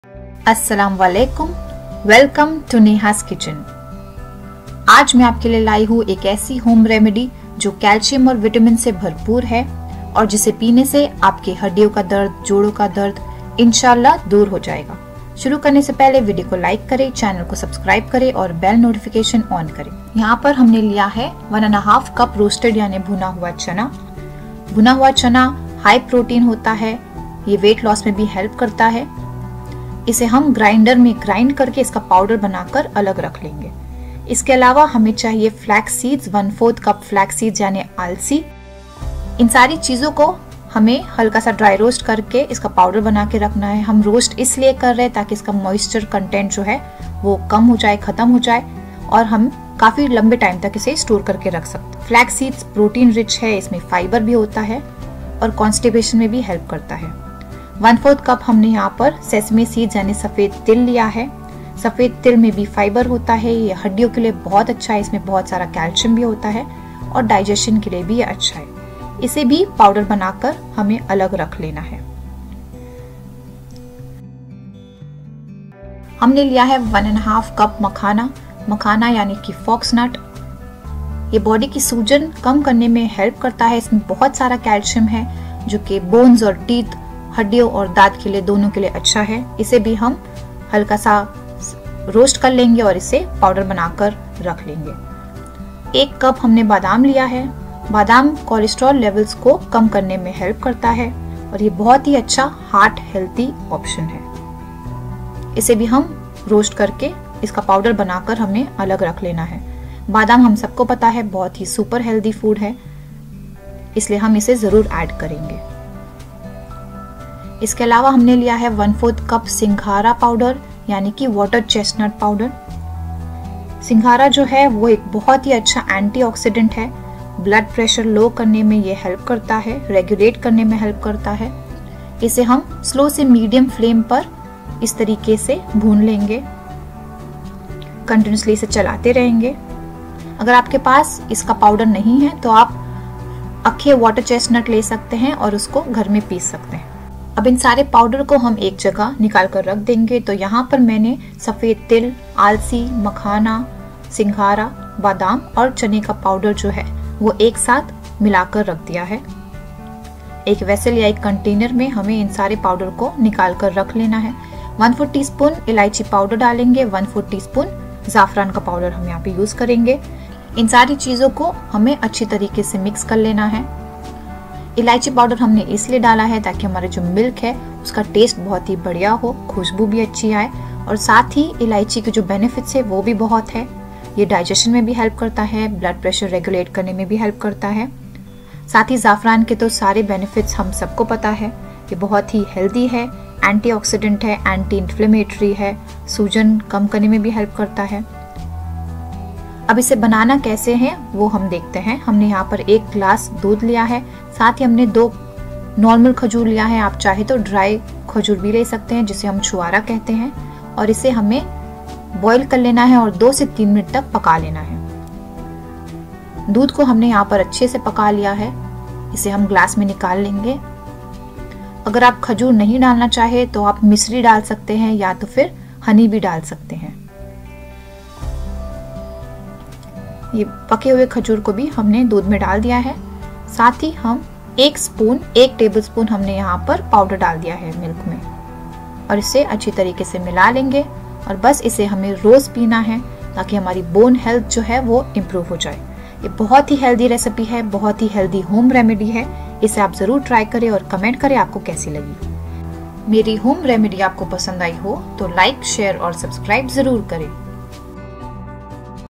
Healthy Home Remedy for Strong Bones | Just 1 spoon daily | Energy Booster | हद्दियाँ मज़बूत करे
Welcome to Neha's Kitchen, your ultimate destination for delicious homemade recipes and culinary inspiration!

Ingredients:
1.5 cup roasted chana
1/4th cup flax seeds (alsi)
1/4th cup sesame seeds (white til)
1.5 cups makhana (fox nuts)
1 cup almonds
1/4th cup singhara (water chestnut) powder